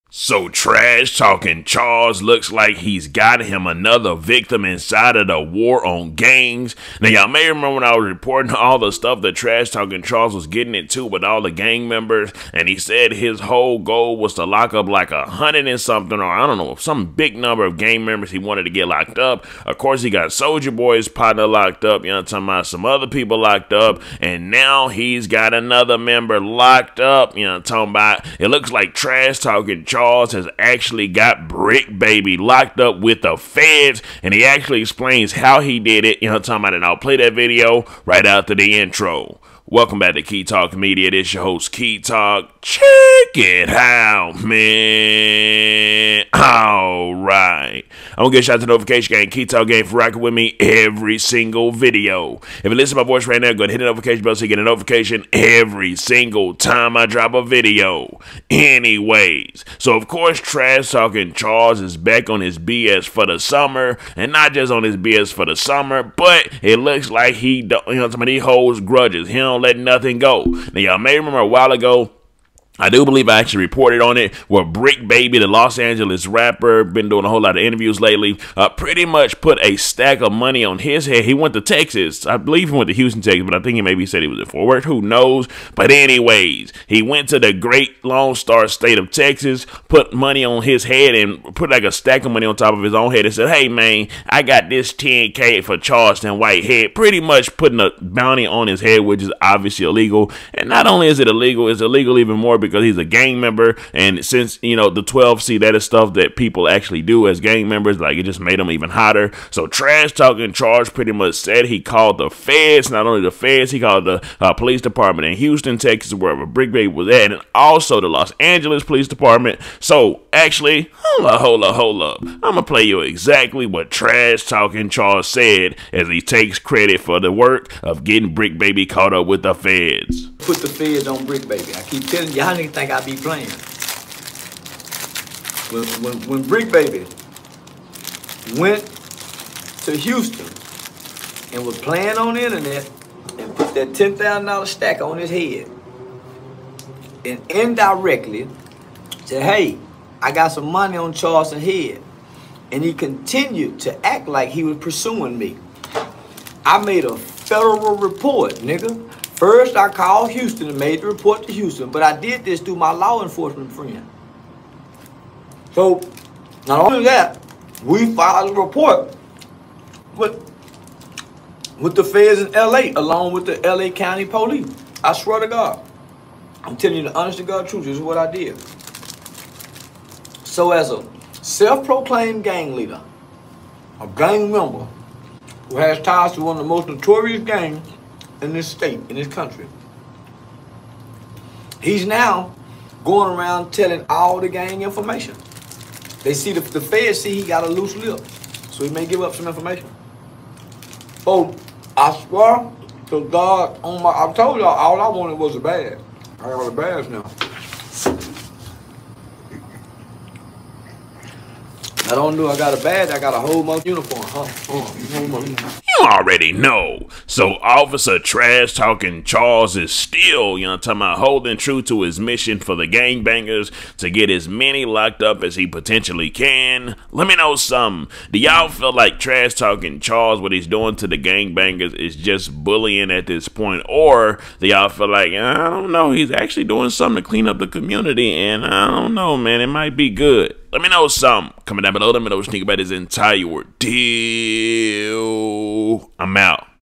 THE WAR & ENSLAVEMENT ON GANGS CONTiNUE! Charleston White CLAIMS WORK 4 Bricc Baby ARREST by FEDS
My podcast!

SCRIPTED! No one was harmed